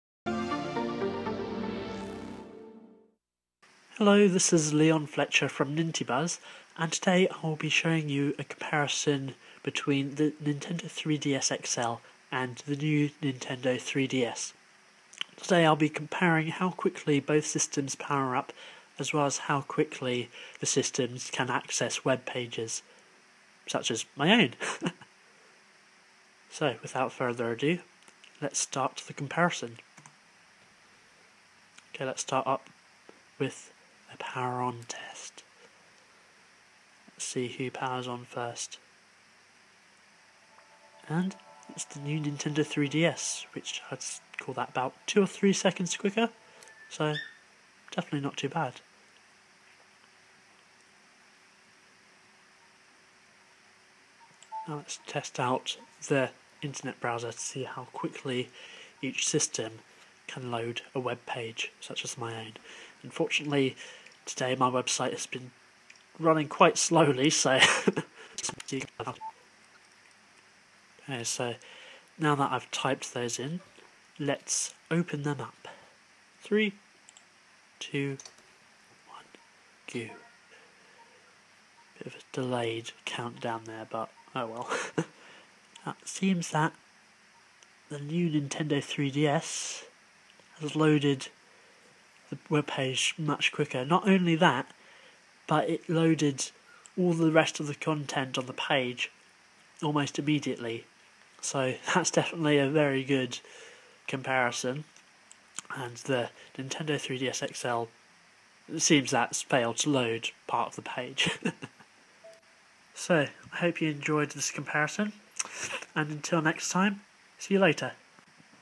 New Nintendo 3DS - Power On/Internet Comparison
Here is a comparison of the powering on time and internet browser connection time between the 3DS XL and the New Nintendo 3DS